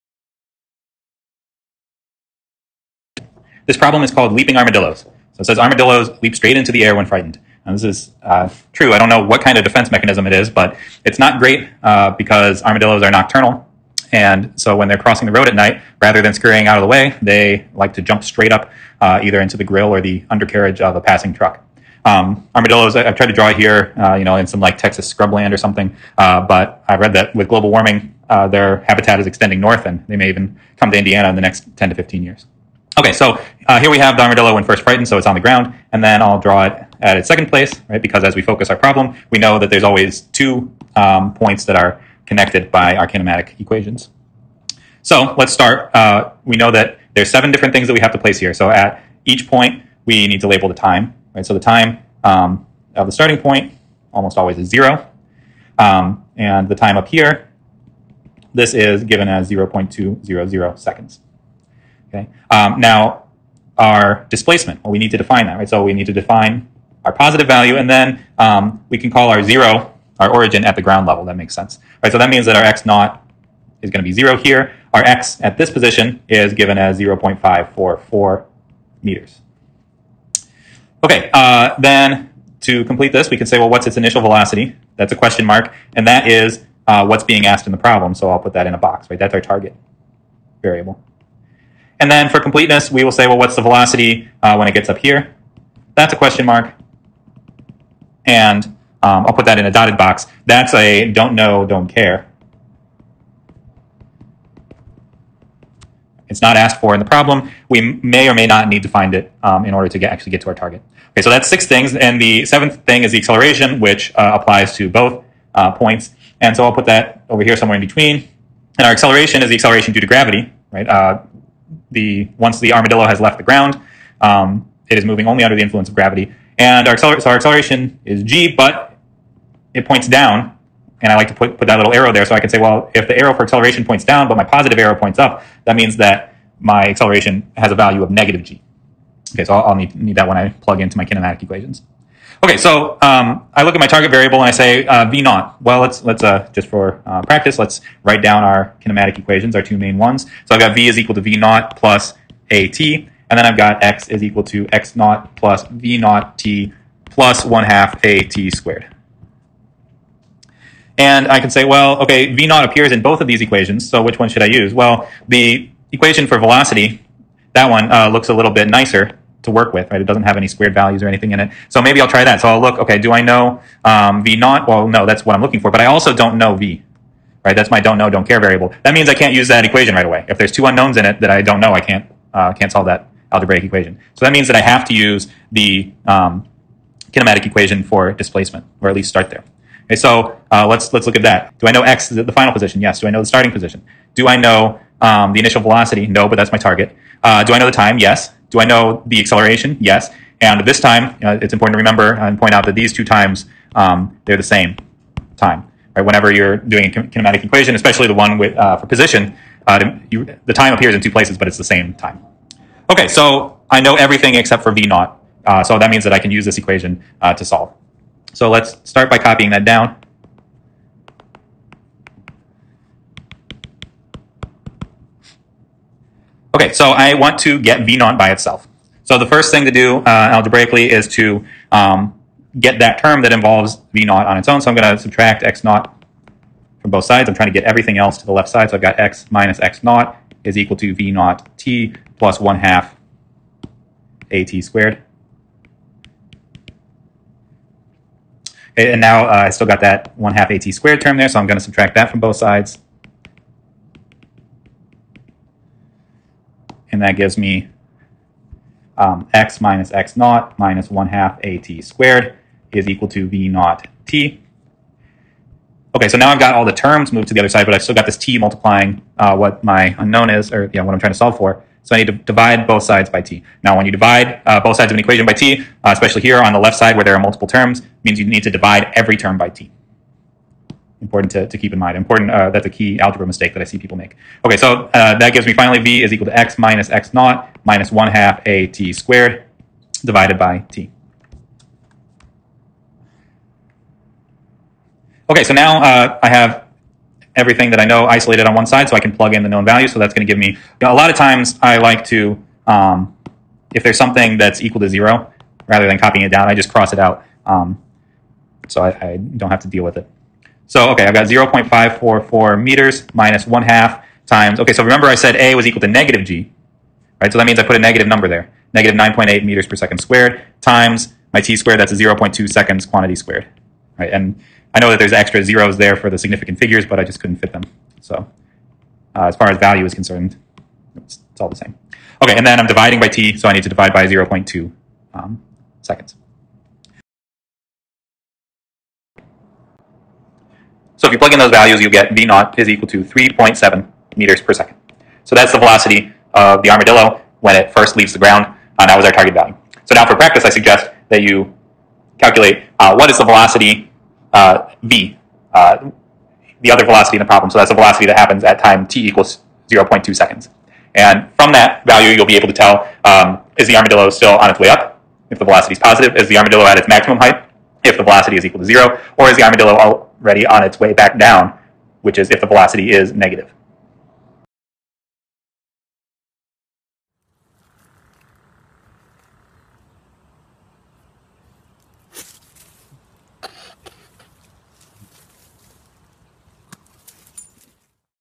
Armadillo jump -- Example of free fall
Free fall problems are just kinematics problems where we don't have to be told the acceleration. We already know the acceleration is due to gravity.

Answer for the challenge problem:
1.04 m/s.